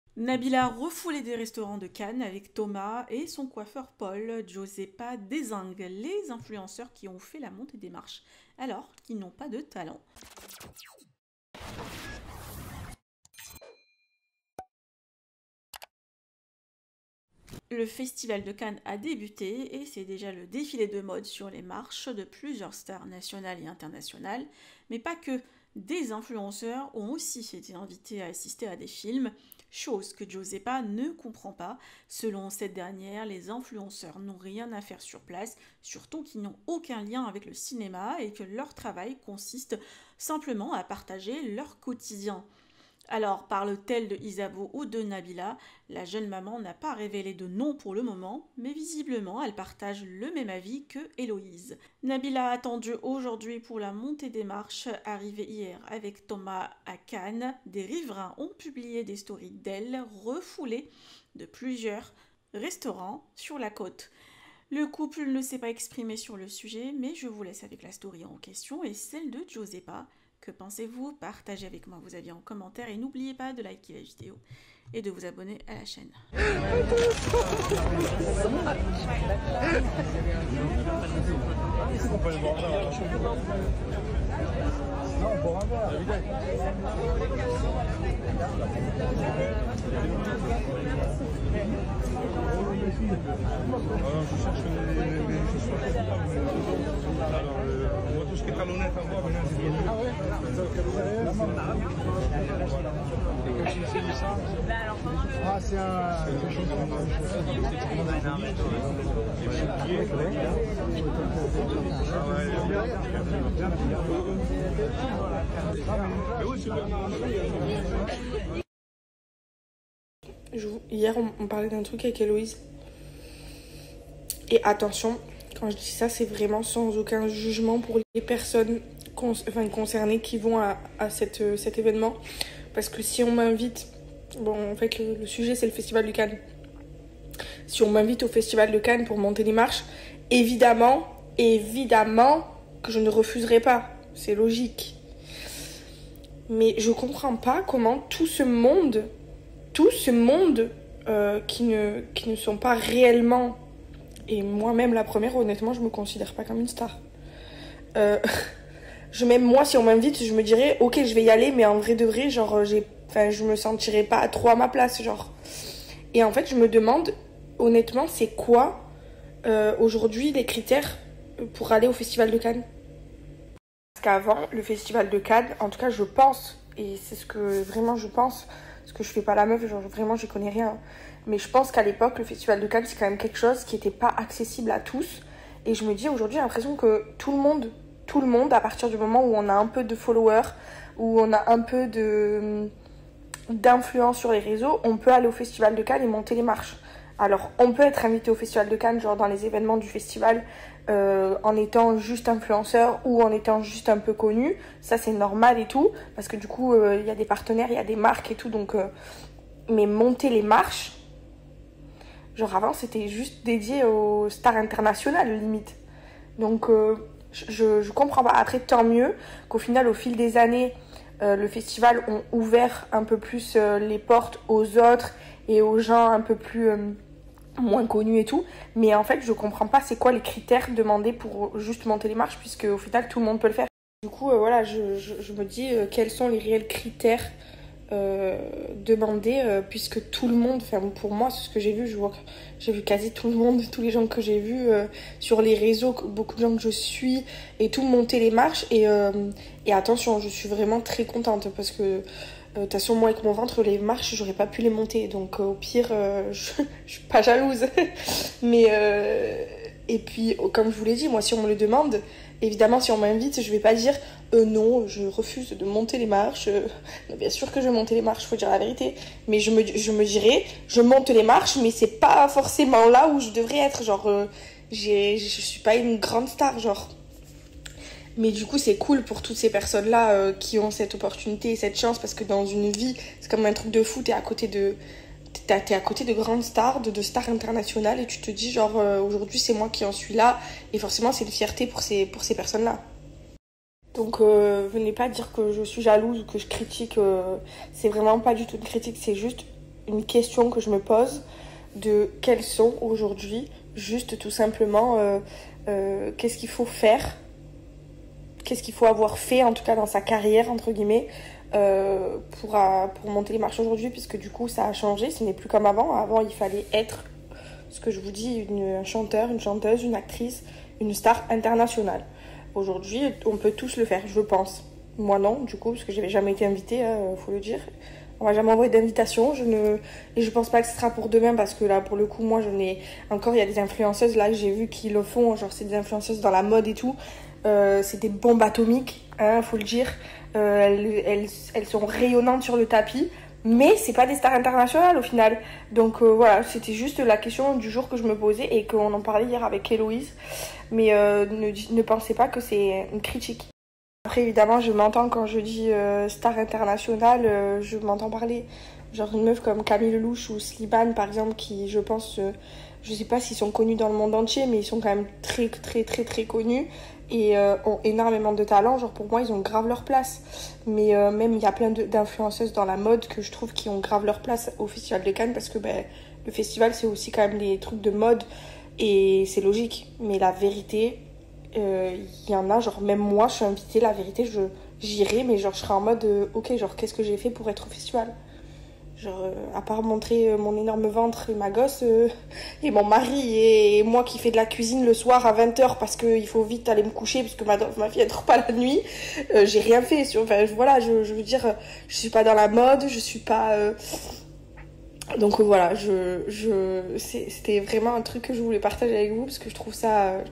NABILLA ET THOMAS VERGARA REFOULÉS DES RESTAURENTS Ä CANNES AUX YEUX DES FANS! GIUSEPPA TACLE FORT
Nabilla Vergara refoulée des restaurants de cannes avec son mari Thomas et son coiffeur Paul? Giusueppa dézingue les influenceuses qui ont fait la montée des marches alors qu'ils n'ont pas de talent